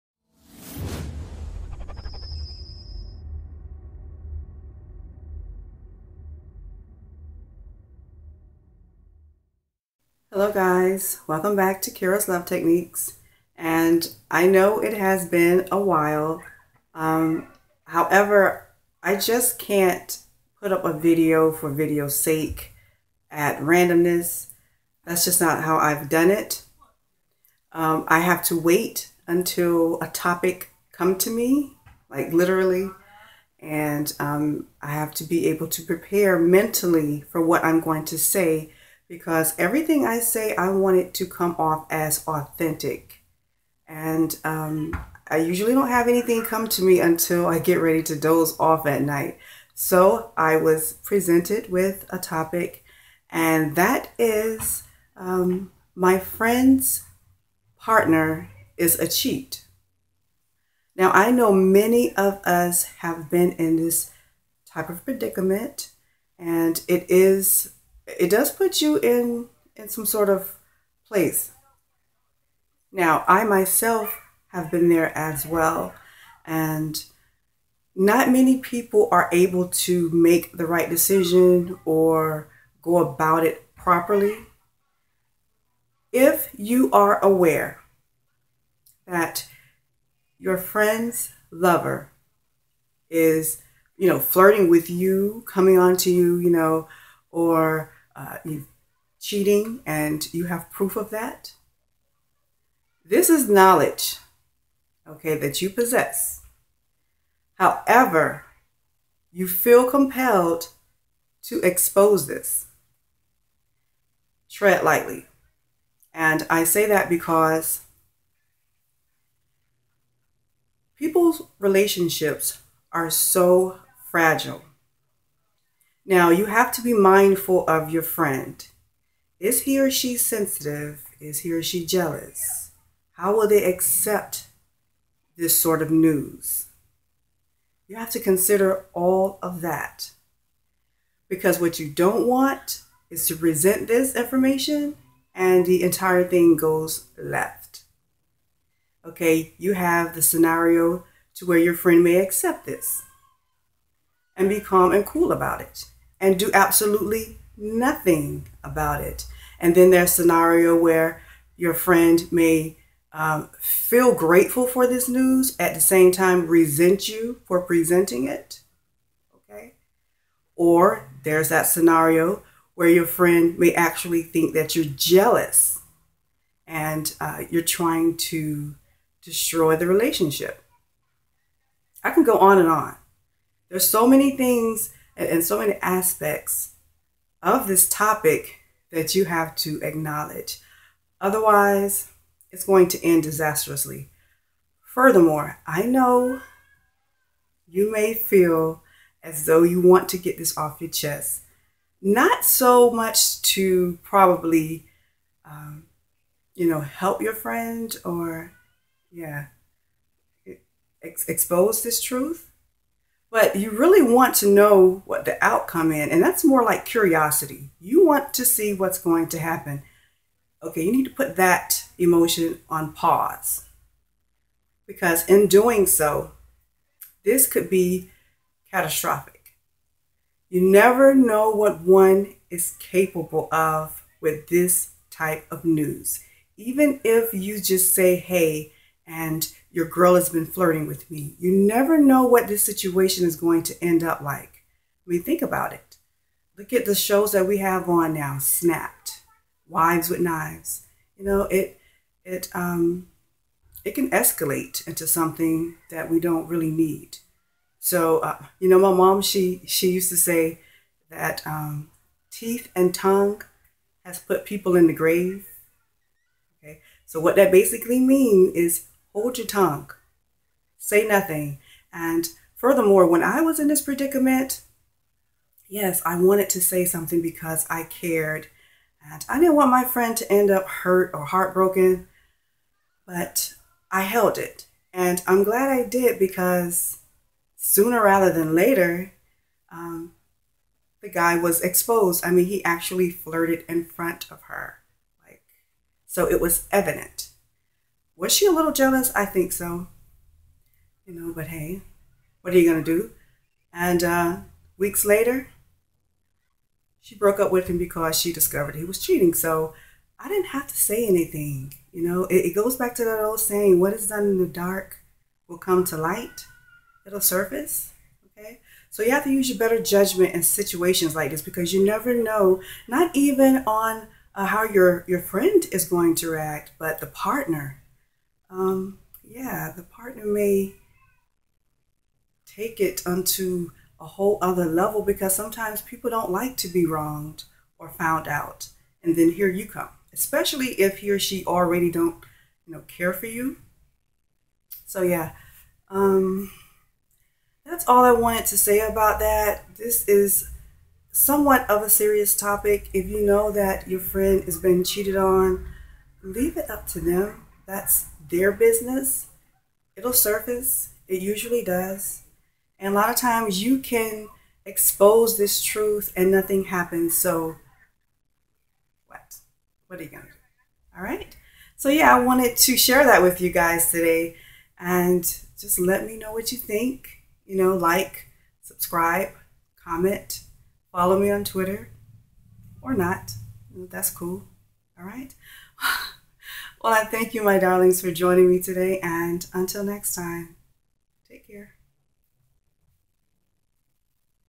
How To Expose A Cheater: Relationship Advice
So you know something that your friend does not?. That his or her lover cheats! I will provide suggestions on how to expose this information effectively.